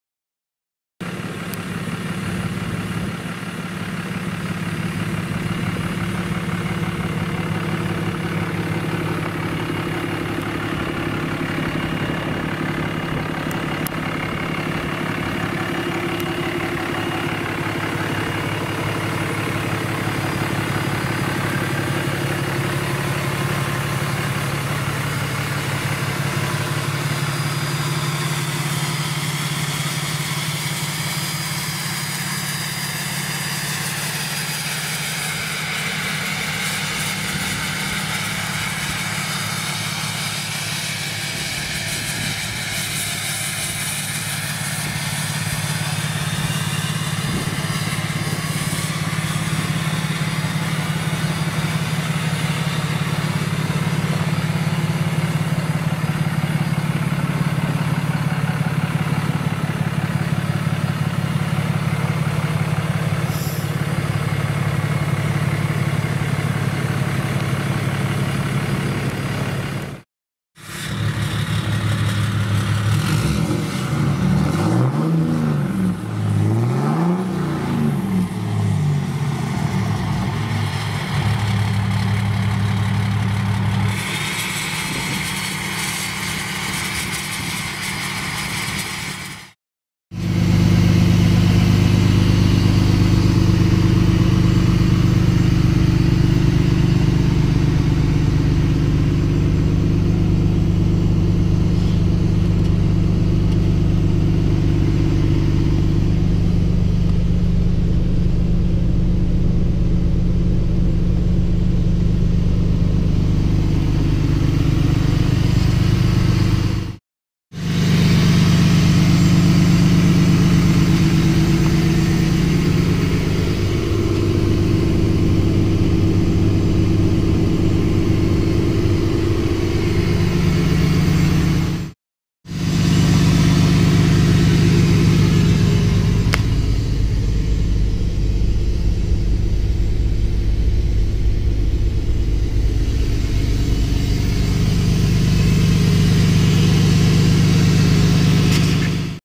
2007 Dodge Ram 2500 Big Horn Cummins 4X4 | Piqua, Ohio 45356
This 2007 Dodge Ram 2500 Big Horn Cummins was for sale in the Piqua, Ohio area. It is no longer available for purchase.